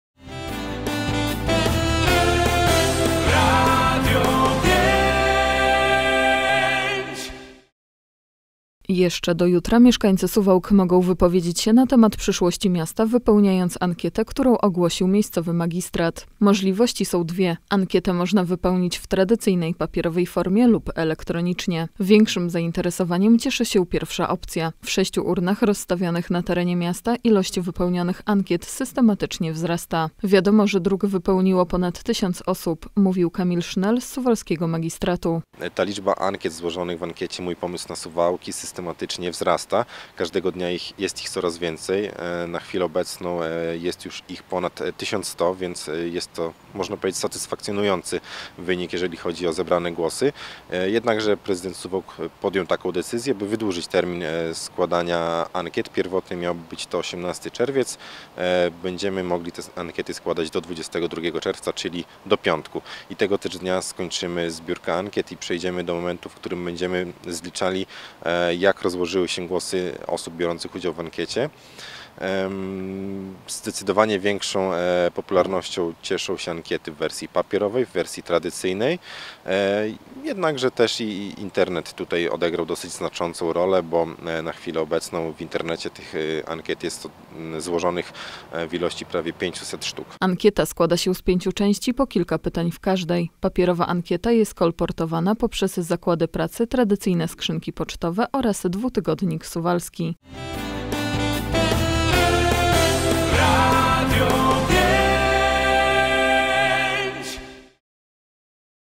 Jutro koniec ankiety
Jeszcze do piątku (22.06) mieszkańcy Suwałk mogą wypowiedzieć się na temat przyszłości miasta, wypełniając ankietę, którą ogłosił miejscowy magistrat. Możliwości są dwie: ankietę można wypełnić w tradycyjnej, papierowej formie lub elektronicznie. Większym zainteresowaniem cieszy się pierwsza opcja. W sześciu urnach rozstawionych na terenie miasta ilość wypełnionych ankiet systematycznie wzrasta.
- Wiadomo, że druk wypełniło ponad 1000 osób - mówi Kamil Sznel z suwalskiego magistratu.